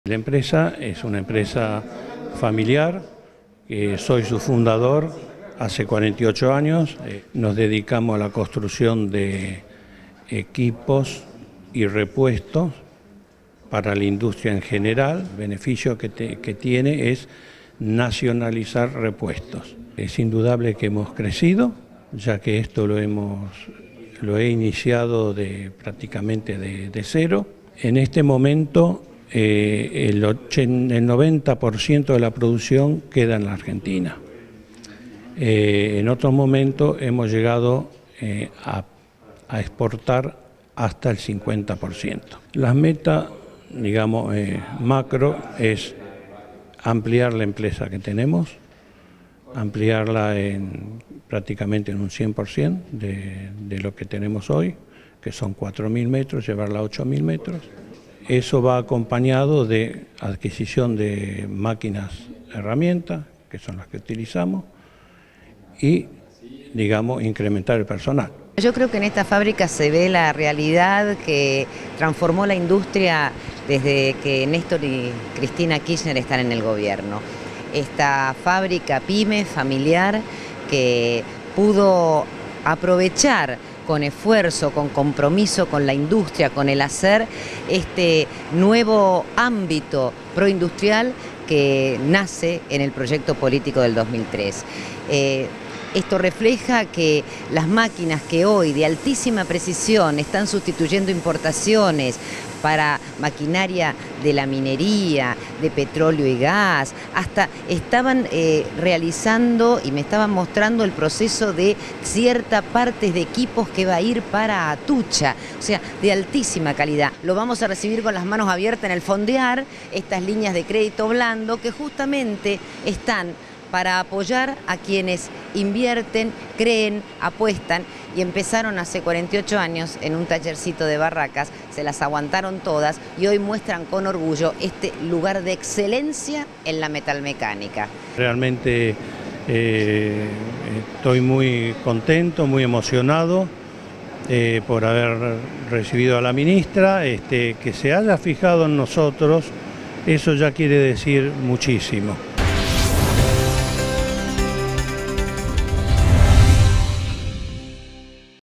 Giorgi: El desarrollo industrial "no dará marcha atrás"
La ministra de Industria, Débora Giorgi, aseguró hoy que el desarrollo industrial argentino “no dará marcha atraś” y dijo que la expansión de las empresas sustituidoras de importaciones se concretó en la última década gracias al impulso del gobierno nacional.
Giorgi formuló estos conceptos al visitar la planta industrial de la empresa metalúrgica Capria, ubicada en el centro industrial de Garín, en el partido de Escobar, en la provincia de Buenos Aires.
La empresa, de mecanizados de precisión, se especializa en la fabricación de repuestos a pedido y entre sus principales clientes se incluyen la industria minera, la siderúrgica, la cementera y la elaboradora de aluminio, entre otras.
La compañía da trabajo a 42 personas entre las que se incluyen ingenieros, torneros y técnicos y ocupa una planta industrial de 5.000 metros cuadrados.
Entre los proyectos de la empresa está la expansión de su planta hasta los 8.000 metros cuadrados y la contratación de mayor cantidad de personal para lo que tiene planeado solicitar créditos.
Al respecto, Giorgi señaló que la compañía podrá pedir recursos al Fondo para el Desarrollo Económico Argentino (FONDEAR), que financia proyectos que promuevan la inversión en sectores estratégicos.
“Esta pyme aprovechó el nuevo ámbito industrial surgido a partir del proyecto gubernamental iniciado en el 2003, con la elaboración de maquinaria de alta precisión, sustituyendo importaciones. Fabrica equipos para la planta nuclear de Atucha, y construye elevadores náuticos”, ejemplificó.
“Esta empresa muestra a la industria real, que duplicó su producción y se lanzó al mundo exportando merced al logro de certificaciones internacionales, control de calidad y nuevos proyectos de diseño”, agregó.
La ministra sostuvo que el dueño de la empresa, Domingo Capria, “lidera la zona industrial del distrito de Garín” y estimó que para la ampliación de su planta seguramente recibirá un crédito blando del FONDEAR.
Asimismo, comentó que la empresa previamente se deberá inscribir en el Registro Nacional de Parques Industriales (RENPI), para poder recibir aportes no reembolsables destinados a plantas industriales.
Domingo Capria, el titular de la empresa, es un tornero de 67 años que comenzó su labor en plena adolescencia en un taller de Barracas, tuvo su primera fábrica en Lanús y luego en la última década se trasladó a la planta de Garín.
Su pyme es de carácter familiar y los integrantes de su familia ocupan distintos cargos en áreas comerciales, financieras y administrativas.
“Hace tres años exportábamos el 50 por ciento de nuestra producción, ahora vendemos al exterior el 10 por ciento y el resto se vuelca al mercado interno”, explicó en diálogo con Télam.
“Tuvimos que competir con la calidad de los repuestos provenientes de Alemania, Francia e Italia, para lo cual insumimos mil toneladas anuales de acero, 40 por ciento de origen nacional y el resto de Italia, China y Brasil”, describió.
“Esta planta de Garín creció un 200 por ciento en los últimos diez años, con una inversión cercana a los dos millones de dólares”, recordó. “Ahora necesitamos tres millones de dólares: dos para ampliar la planta y uno para adquirir más maquinarias”, calculó.
Dijo que la ubicación de la planta en Garín facilita su actividad porque la zona es un “corredor estratégico para hacer entregas a tiempo” a clientes ubicados en las inmediaciones como la empresa Techint de Campana.
“Nosotros con esta empresa empezamos de cero. Fuimos constantes. No tuvimos saltos y reinvertimos en maquinarias”, narró.
Entre los principales clientes de la empresa se incluyen compañías como Siderca, Siderar, Metal Mecánica, Aluar, Minera La Alumbrera y la mexicana TAMSA, especializada en tubos de acero, entre otras.
“Nuestra empresa es única porque elabora productos a pedido, no tiene línea de producción”, explicó Capria.
La empresa participa activamente del Plan de desarrollo de proveedores y sustitución de importaciones para la Industria Minera de la Argentina que lleva adelante el Ministerio de Industria de la Nación.
Algunos de los desarrollos que realiza para el sector minero son bujes y ejes especiales; eje motriz con rueda dentada; cuerpo de bombas; cadenas porta cangilones y apoyo de horno, con los que provee a grandes compañías como Minera Alumbrera, Barrick y Cerro Vanguardia.
Además es fabricante de bienes de capital y proveedora de bienes y servicios a otro sector estratégico como la industria del gas y petróleo.
Exporta desde 1996 a diversos destinos como Chile, Canadá, Estados Unidos, Paraguay, Perú, Venezuela, México, Brasil e Indonesia.